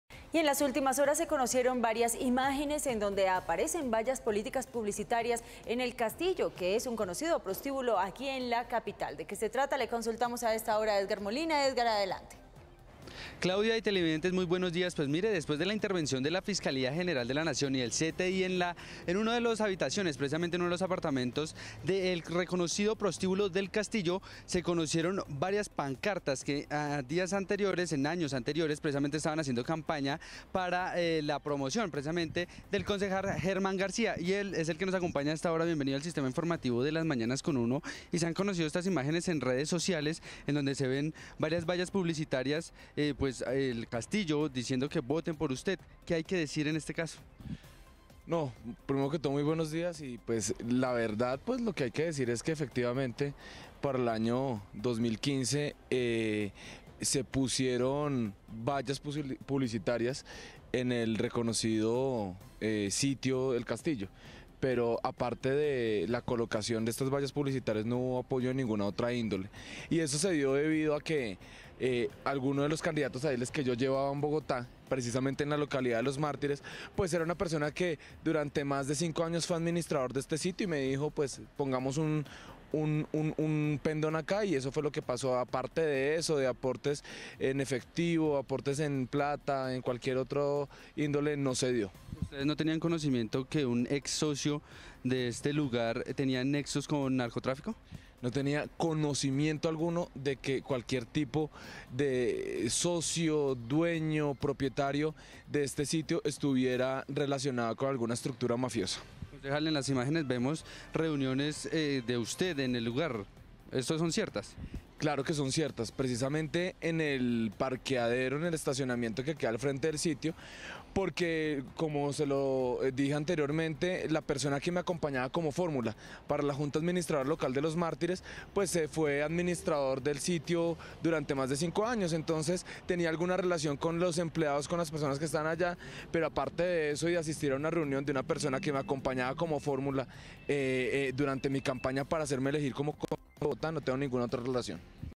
Denuncian vallas políticas en prostíbulo de Bogotá
Se conocieron varias imágenes en donde aparecen vallas políticas publicitarias en El Castillo, conocido prostíbulo de Bogotá.